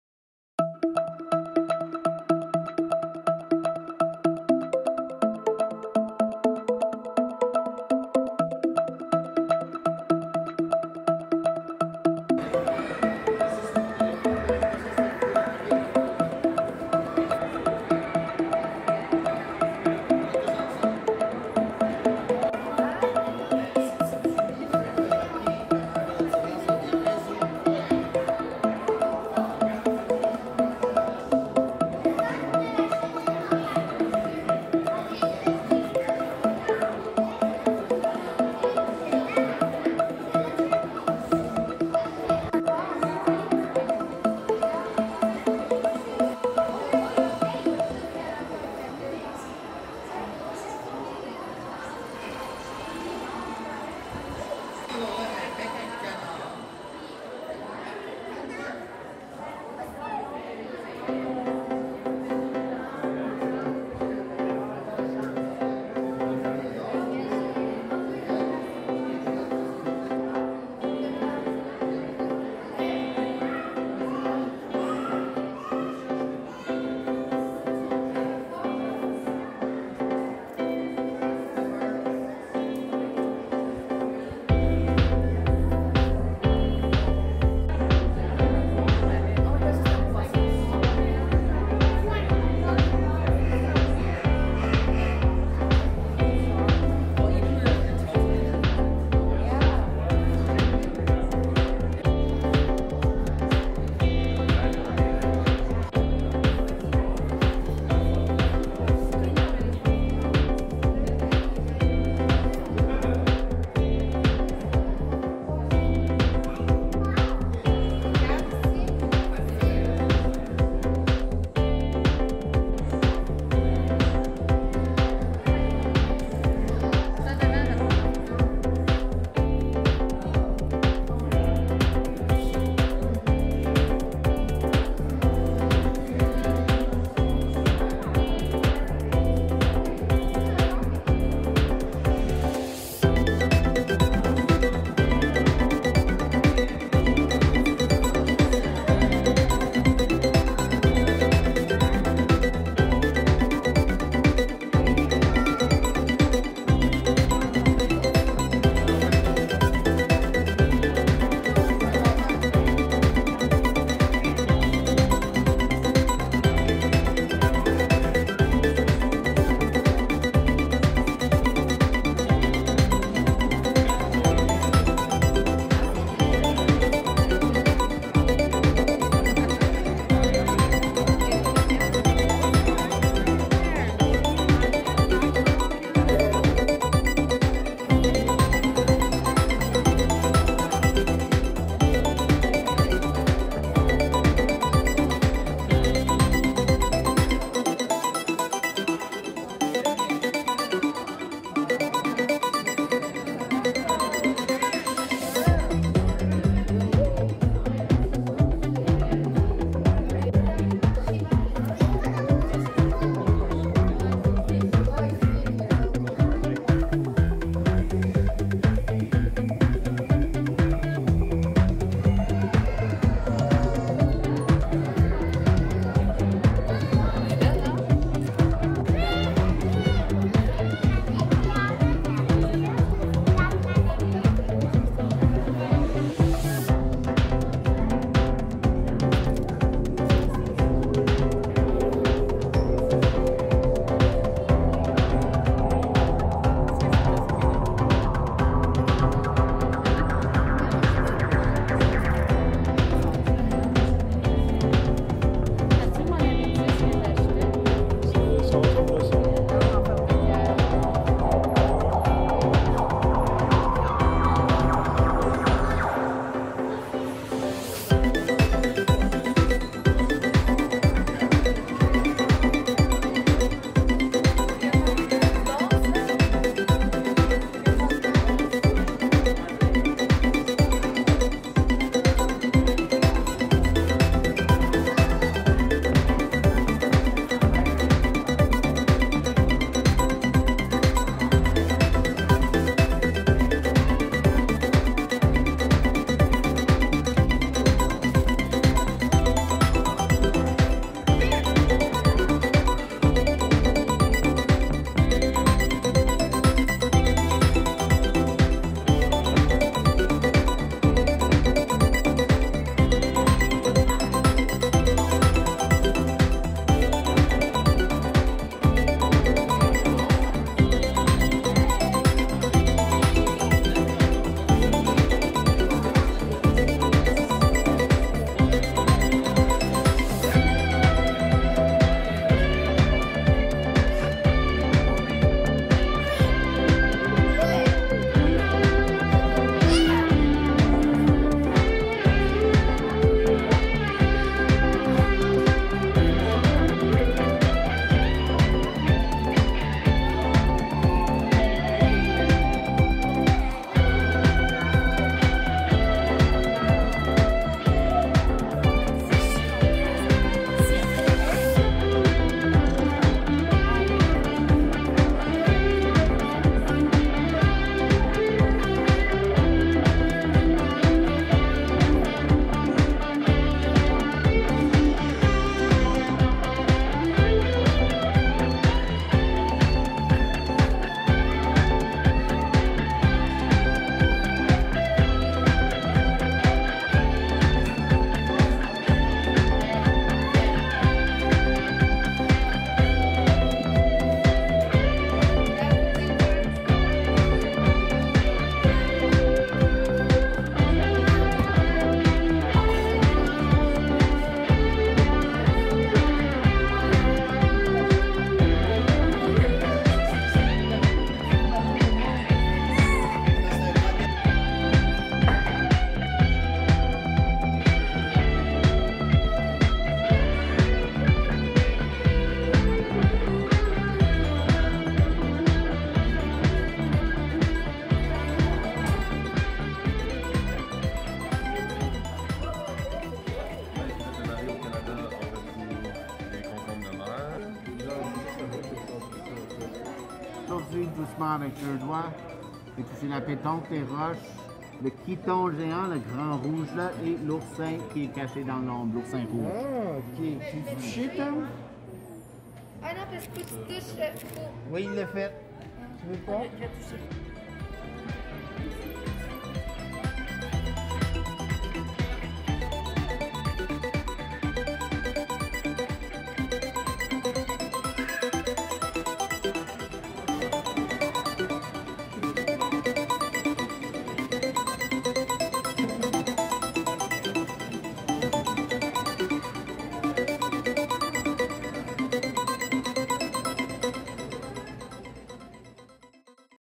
Musée de la nature canadien
Le Musée canadien de la nature est un musée d'histoire naturelle d'Ottawa au Canada, situé à l'intersection des rues Metcalfe et McLeod. Ses collections furent initiées par la Commission Géologique du Canada en 1856.